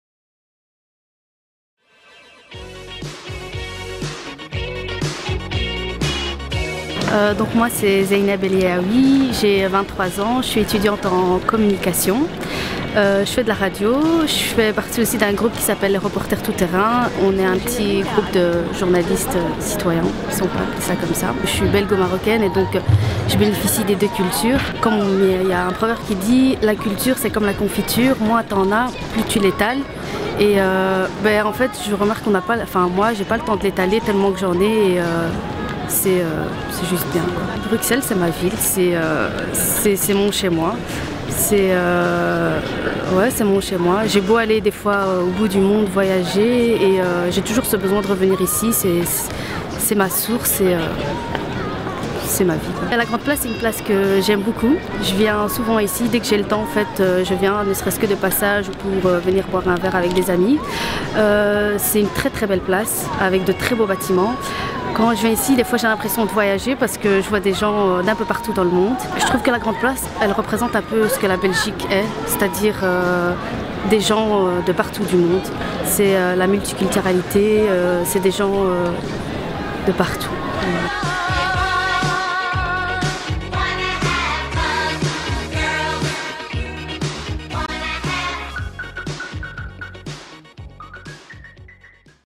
Zaineb El, bruxelloise & voilée.
Elles sont bruxelloises & elles sont voilées.

Bruxelloises & Voilées pour déconstruire les préjugés.